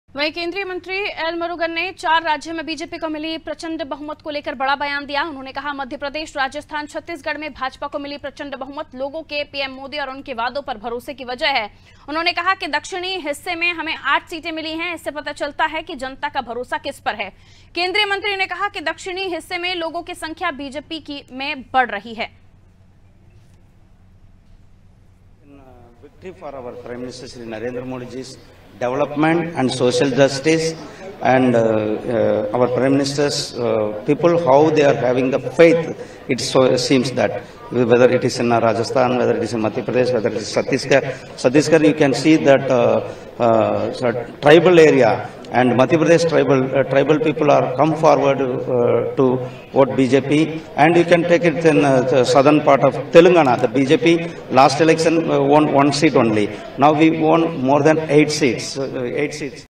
Delhi : केंद्रीय मंत्री एल मुरुगन का बयान, चार राज्यों में मिली प्रचंड जीत पर बोले || Anaadi Tv
AIRTEL - 379